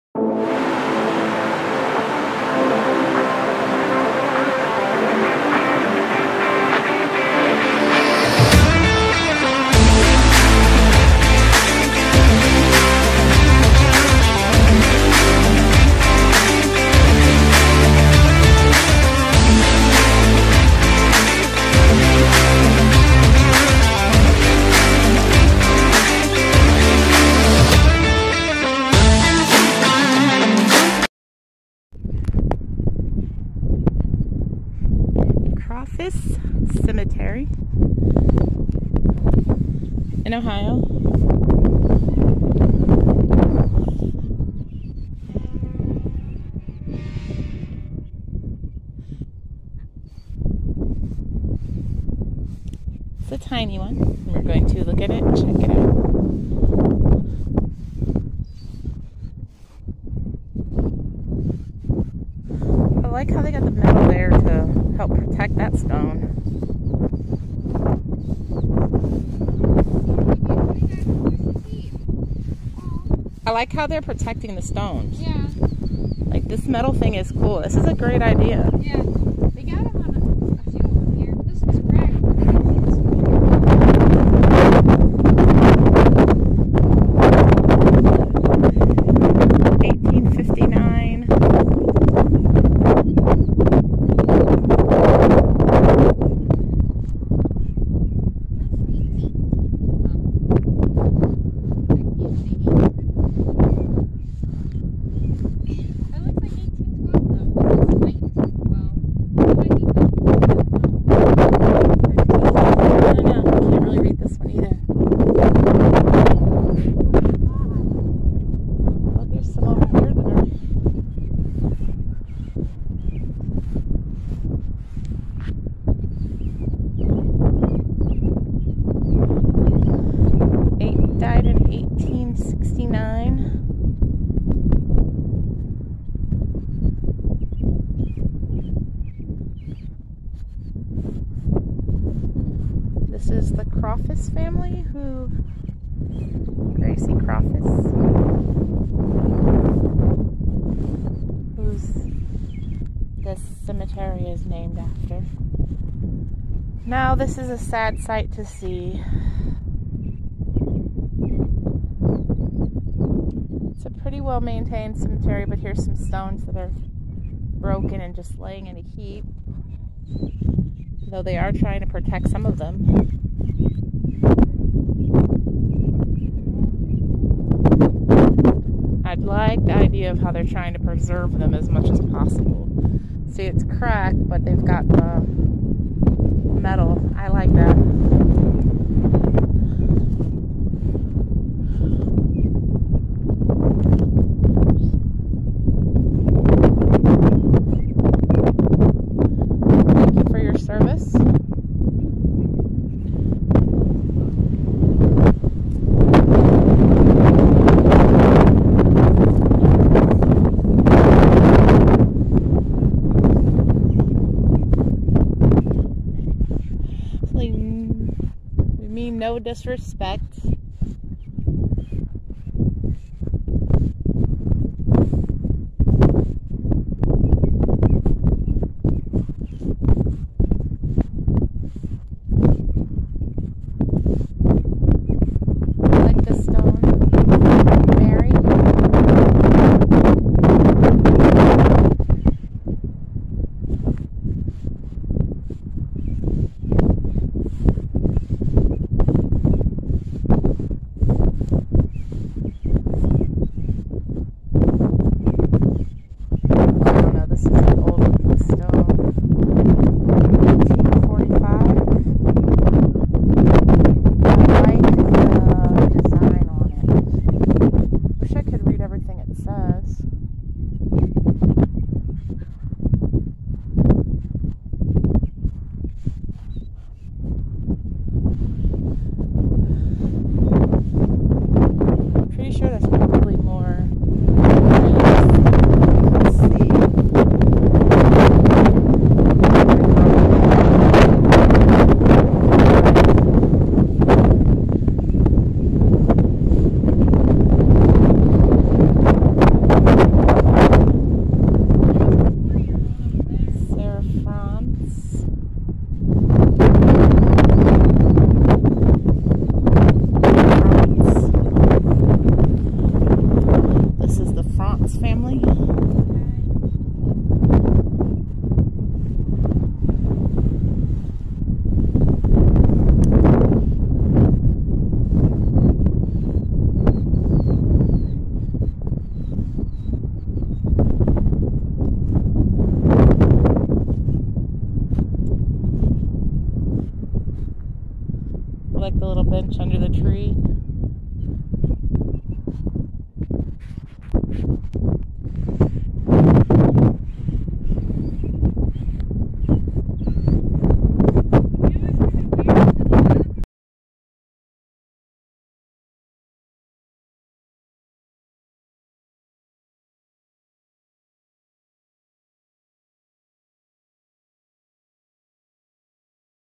Crawfis
This little cemetery seems to be a family cemetery.